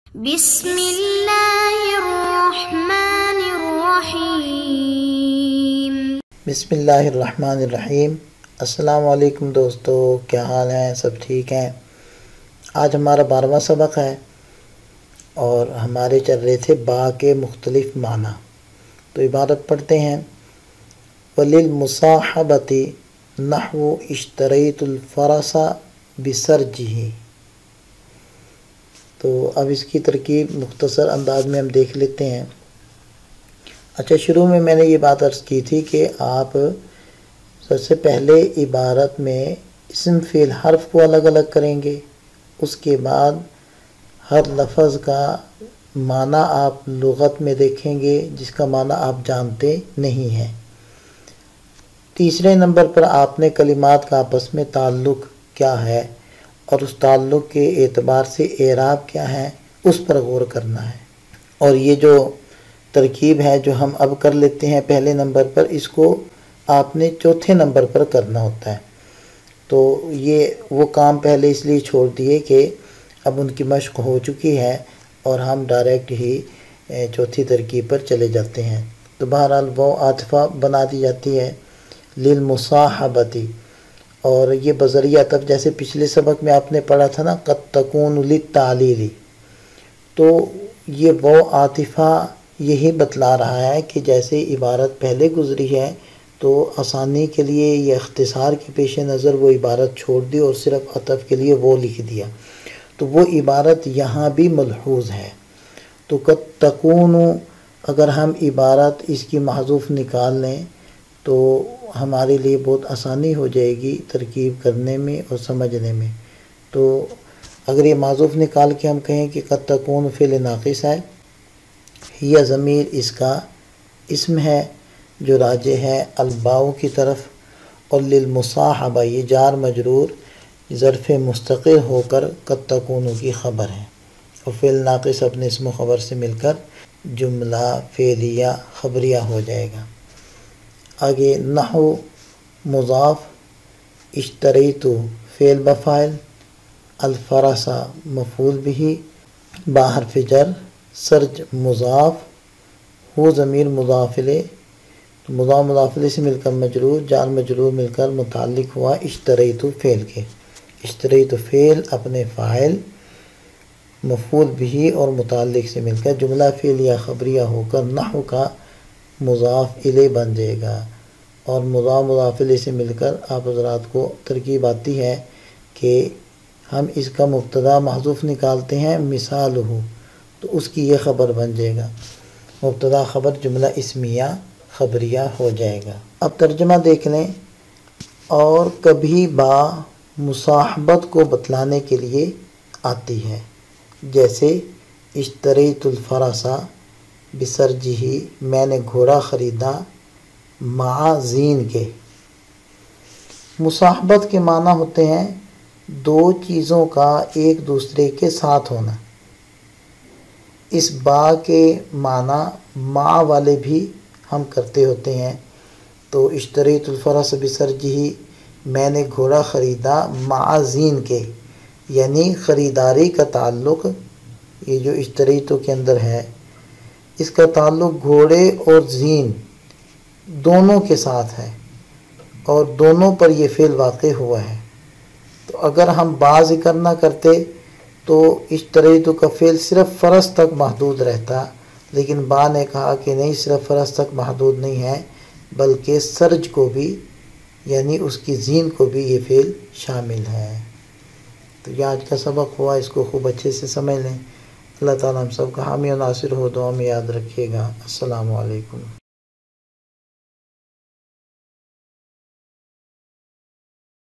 شرح مائۃ عامل سبق 12 الباء للمصاحبۃ سے بسرجہ تک
"علم النحو آسان اور عام فہم انداز میں
شائقین کیلئے نادر موقع مدرسین اور
 طلباء کیلئے یکساں مفید"